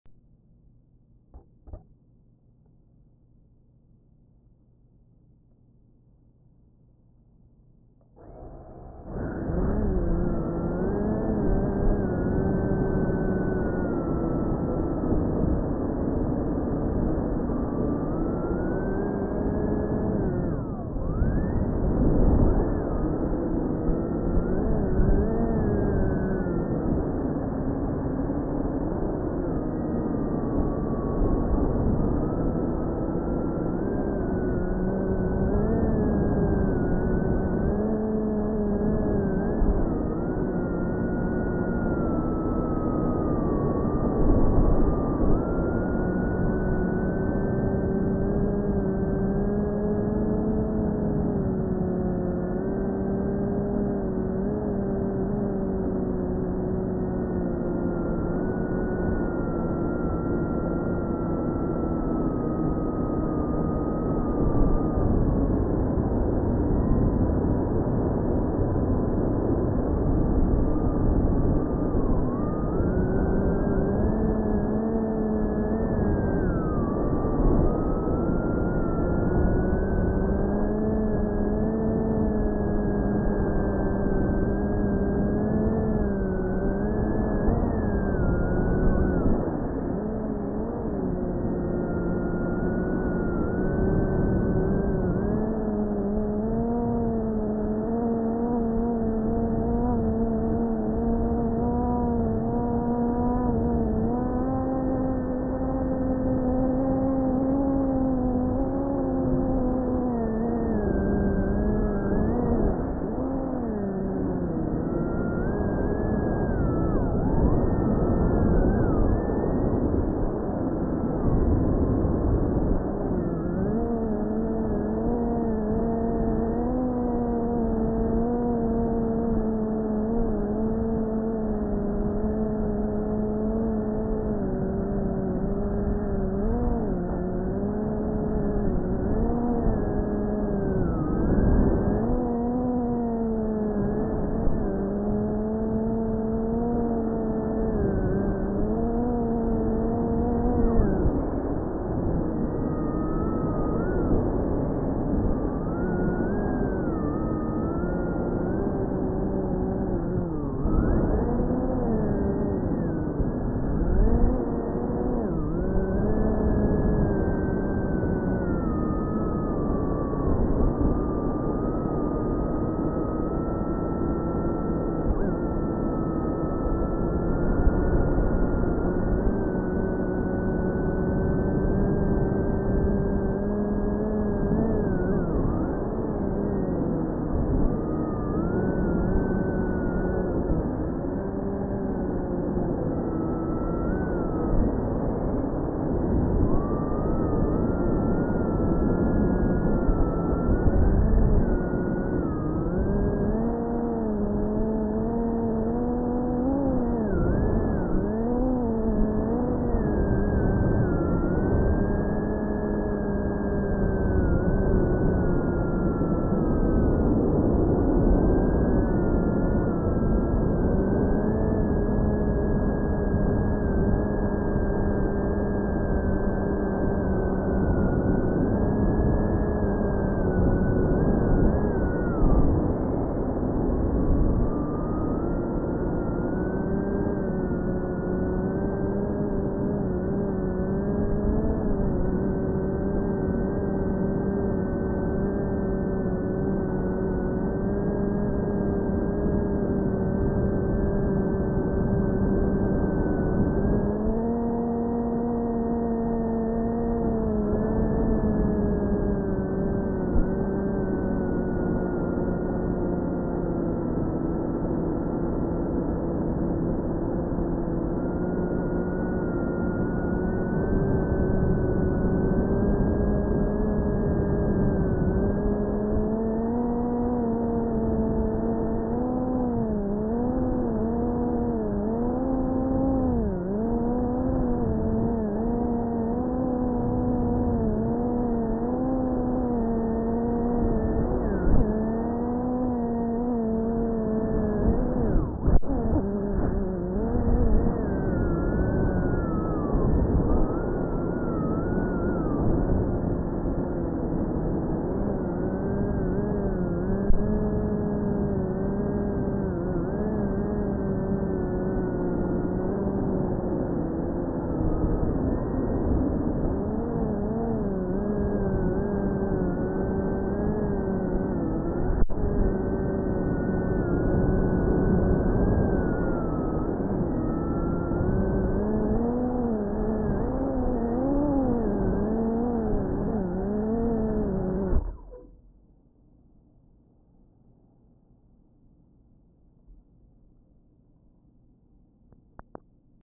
Night Flight // RUNCAM THUMB PRO
ISO : 1600
SHUTTER: 1/60
Low contrast/Low sharpness
Raw footage, non stabilized, non color corrected
3 inch cinewhoop
4S Batt

you aint need no gopro to fly well